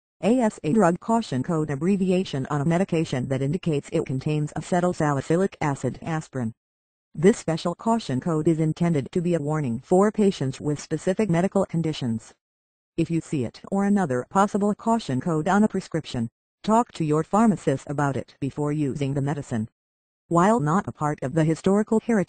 What Is The Definition Of ASA drug caution code Medical Dictionary Free Online
what is the definition of ASA (drug caution code): Abbreviation on a medication that indicates it contains acetylsalicylic acid (aspirin).

This special caution code is intended to be a warning for patients with specific medical conditions. If you see it or another possible caution code on a prescription, talk to your pharmacist about it before using the medicine.

While not a part of the historical heritage of ancient prescription abbreviations, drug caution codes provide very valuable warnings.